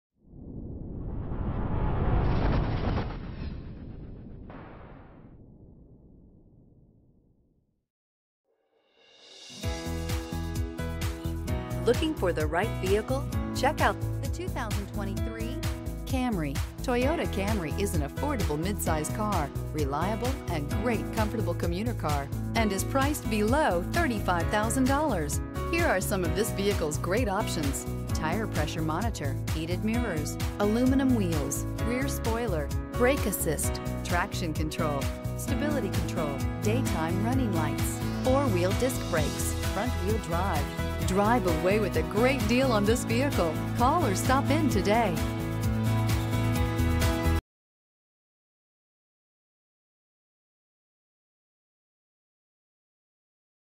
2023 Toyota Camry Metairie LA PL3133
2023 Toyota Camry SE

Lakeside Toyota
3701 North Causeway

Black 2023 Toyota Camry SE 2.5L I4 DOHC 16V 8-Speed Automatic28/39 City/Highway MPGBlk Cloth.